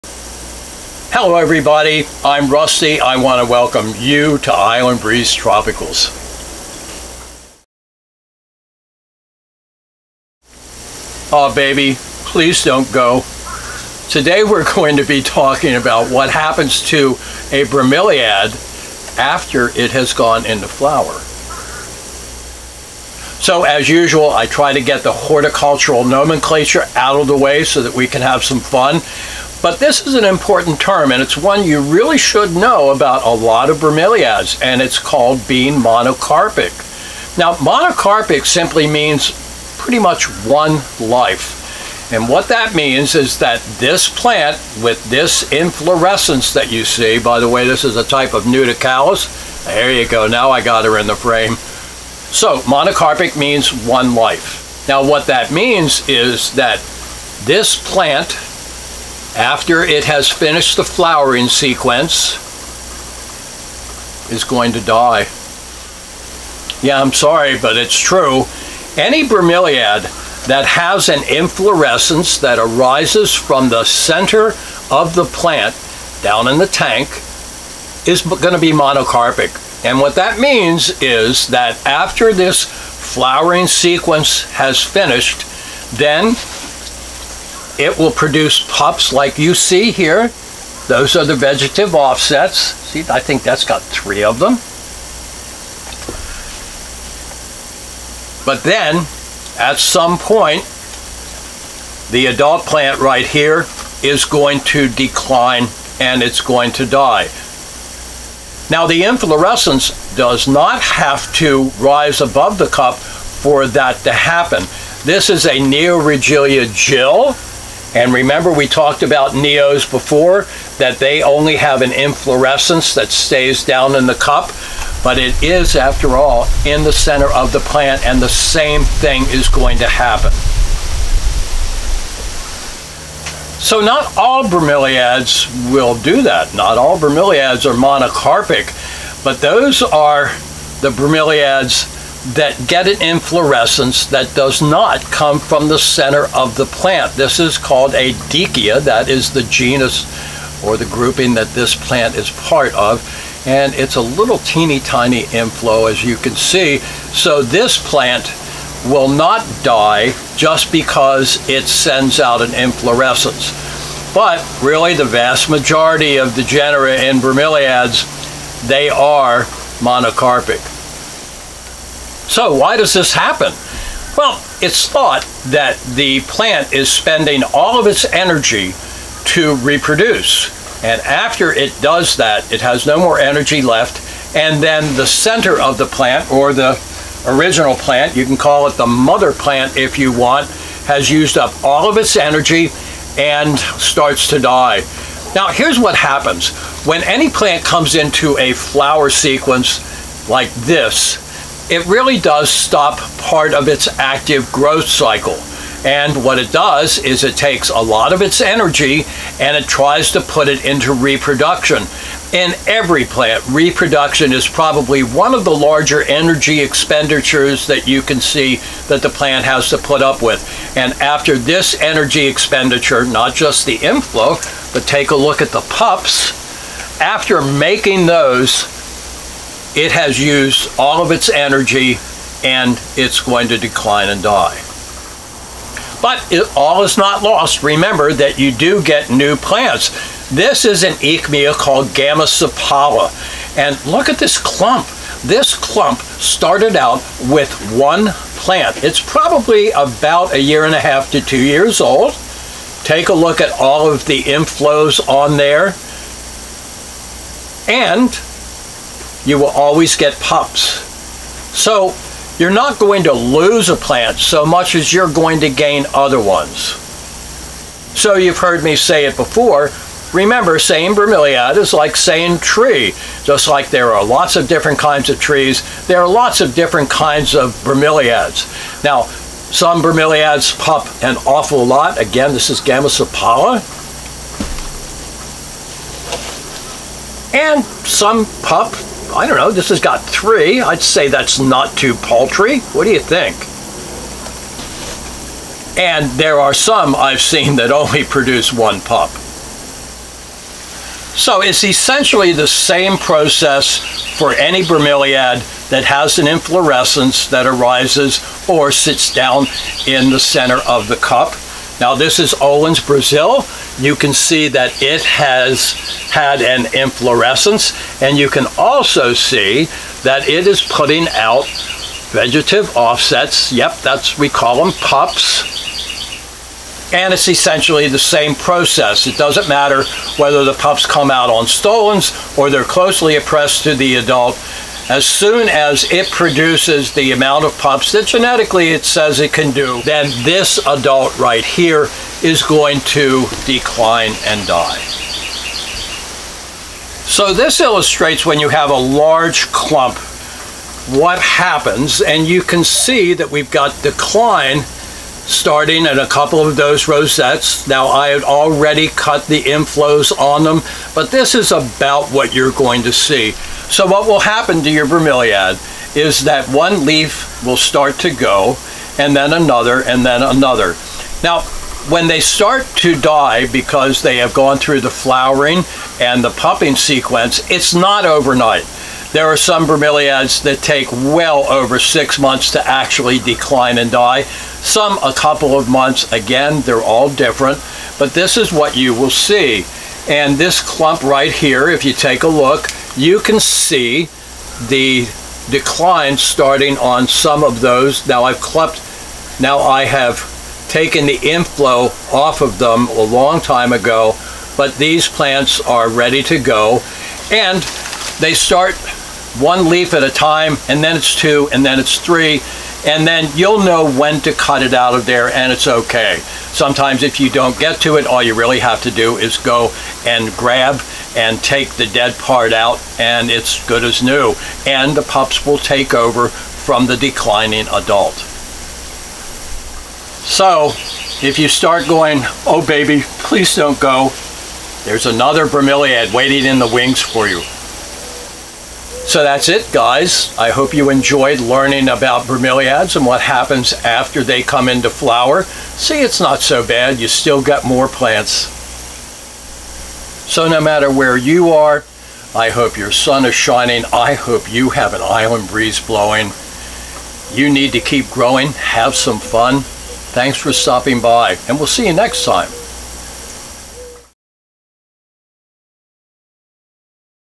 Do Bromeliads DIE after they flower?
Do Bromeliads DIE after they flower?  In todays PlantVlog we'll look at why a lot of bromeliads die after flowering, and why it isn't necessarily a huge loss. It's called being monocarpic, and most bromeliads do it. Learning all about monocarpic bromeliads is really easy, …so come on, ‘Let’s get growing and have some fun!